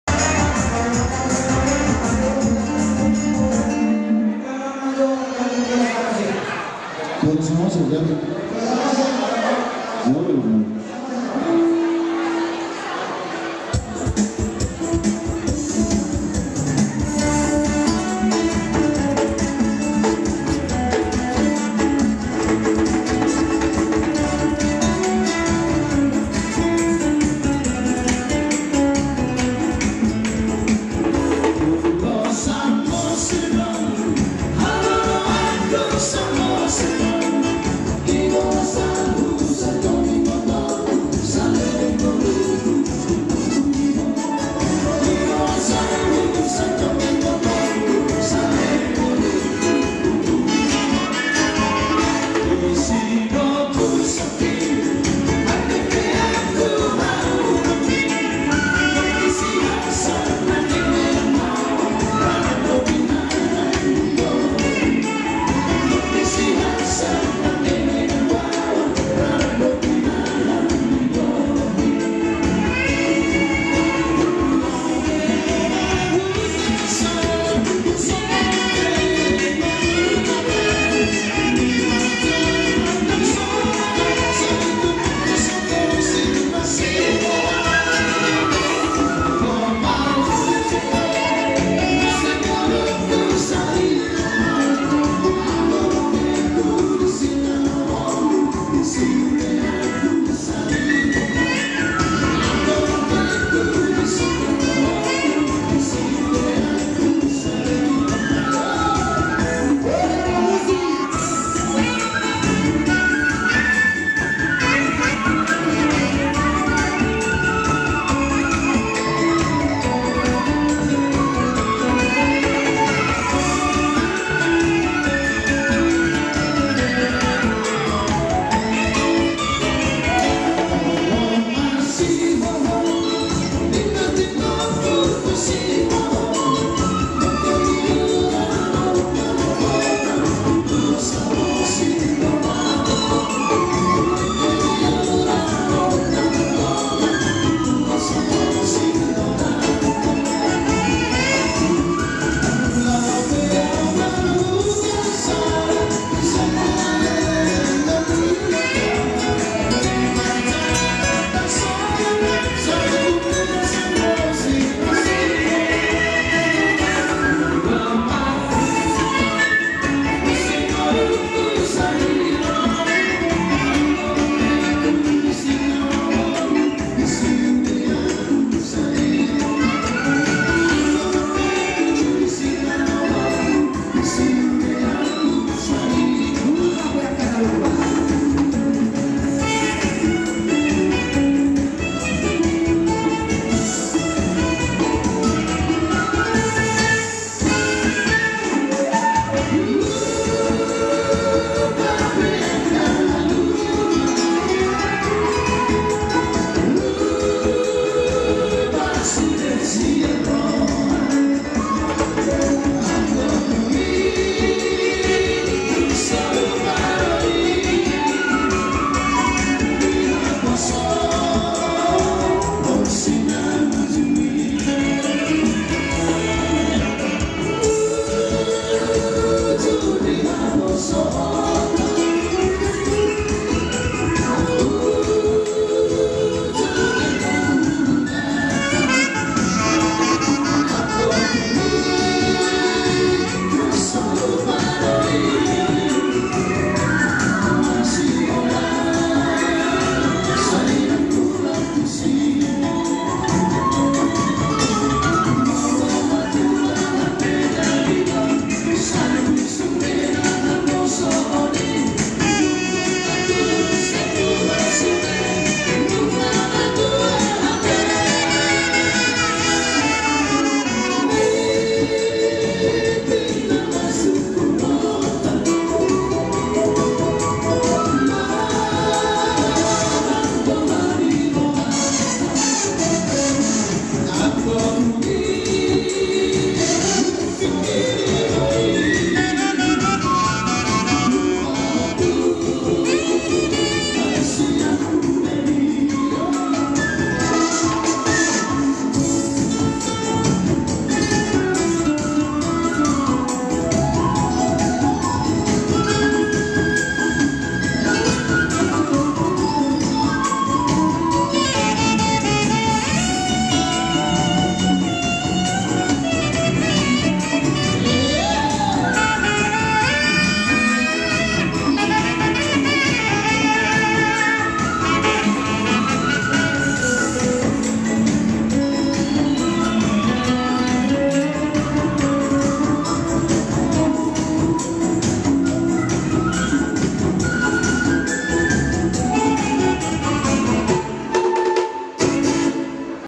WeddingBatak Kel:Pasaribu&Kel Marbun Bersama Grup Bona Rio Musik Di Gedung Graha Bintang
BONA RIO MUSIK MOHON DISUBSCRIBE YA